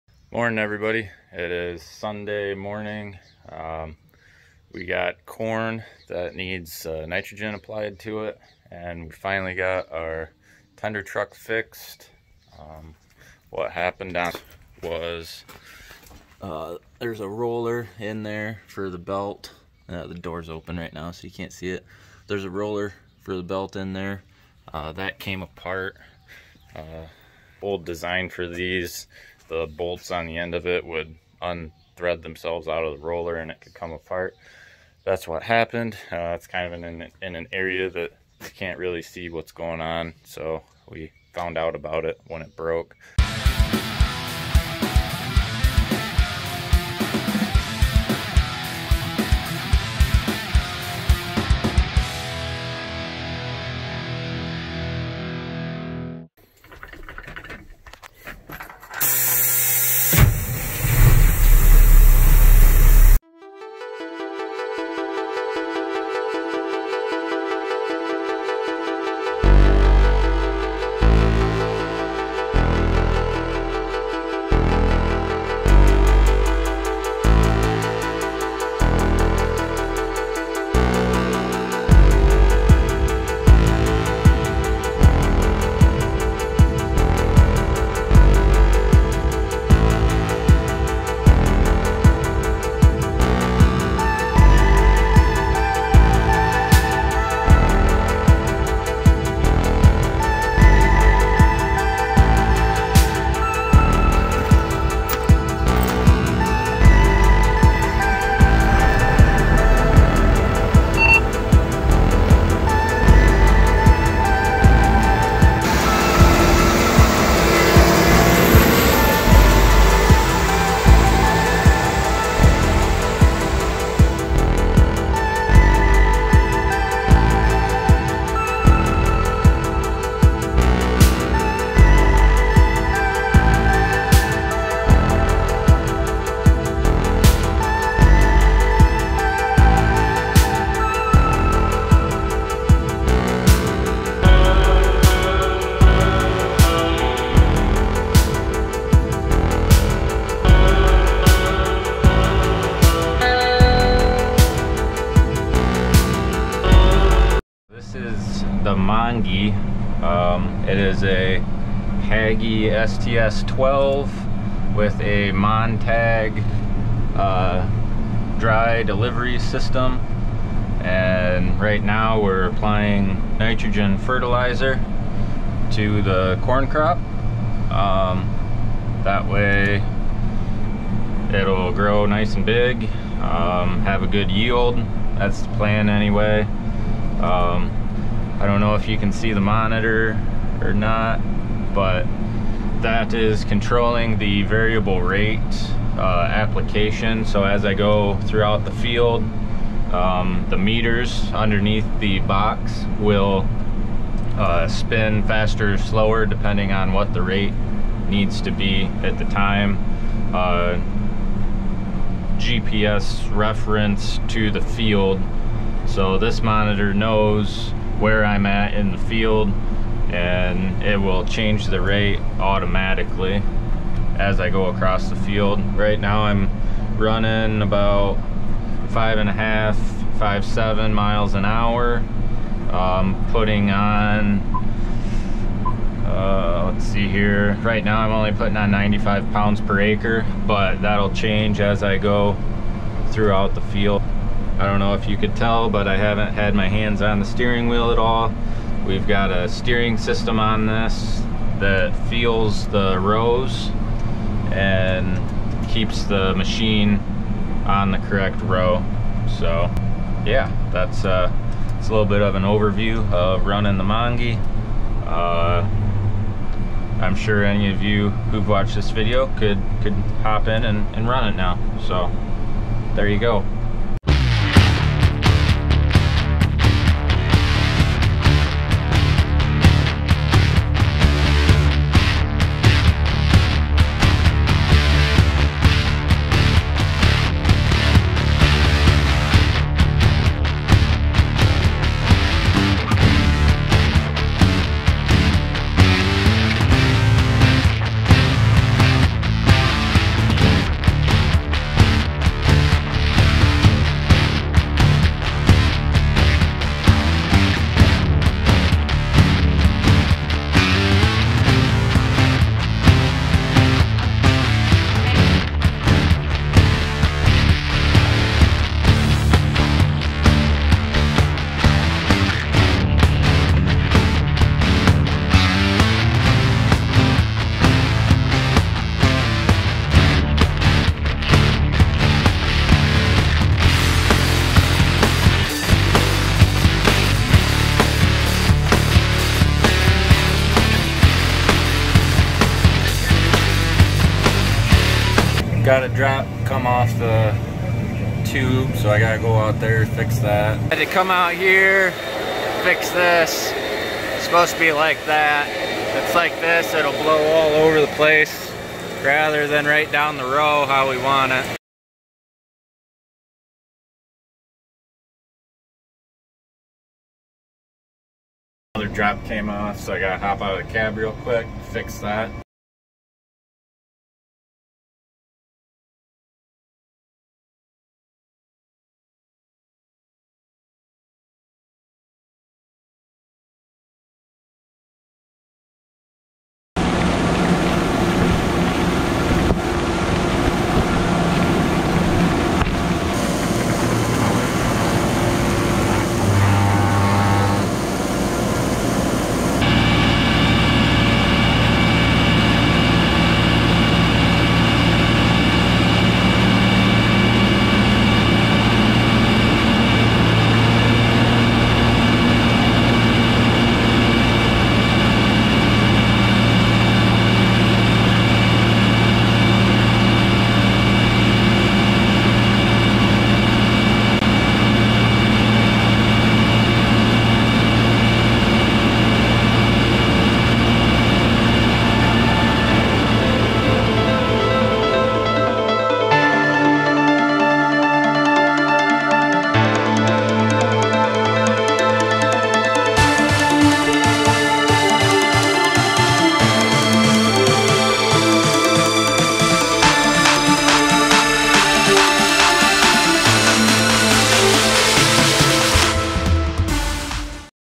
HAGIE STS 12 | Montag Fortifier on 120' Boom | Applying Nitrogen
This video highlights our Hagie STS 12 we call "The Mongie". With a Montag air delivery system on a Hagie frame this Hagie becomes a whole new machine. In this video it is applying nitrogen to the corn crop but can also be used to seed cover crops. Thanks for checking it out!